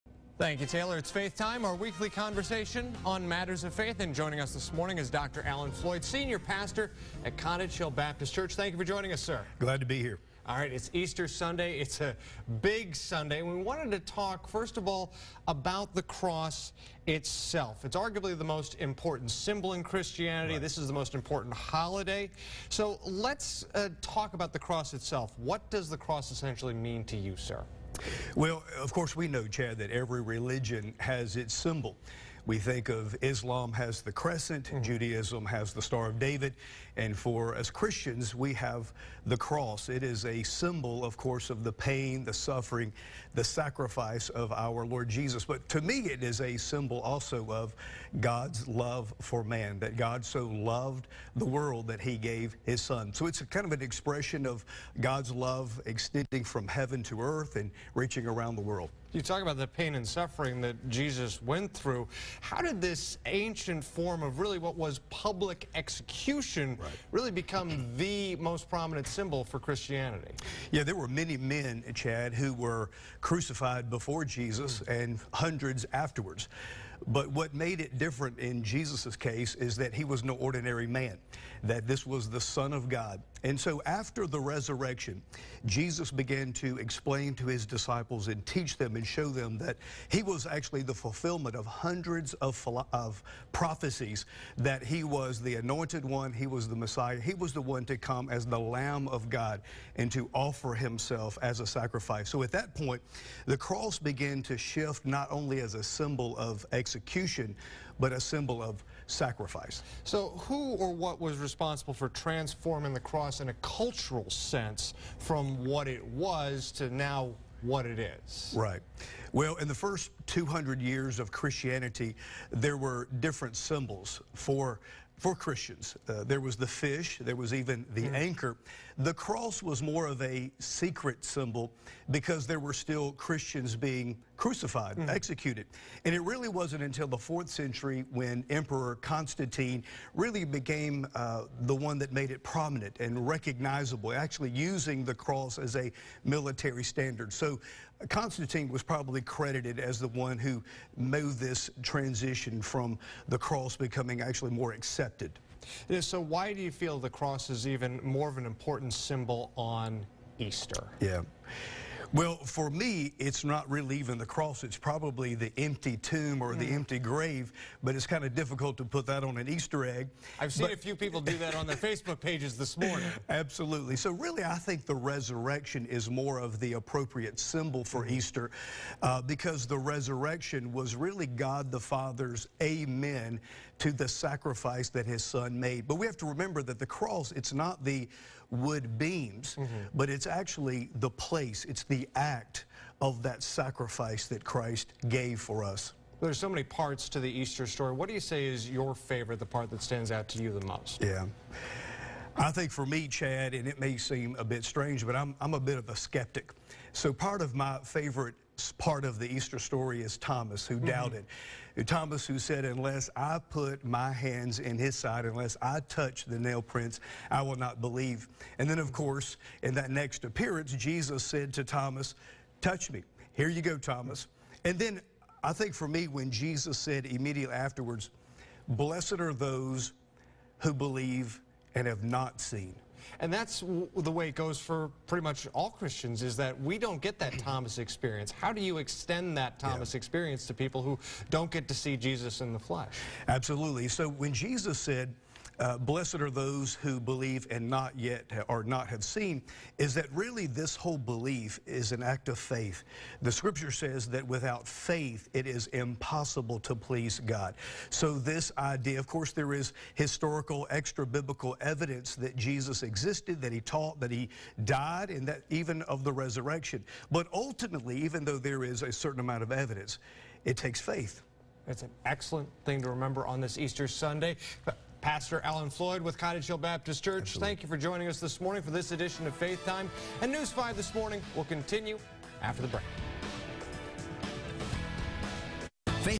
Faith Time: Easter 2018
Dr. Alan Floyd with Cottage Hill Baptist Church joins us to talk about the symbol of the cross.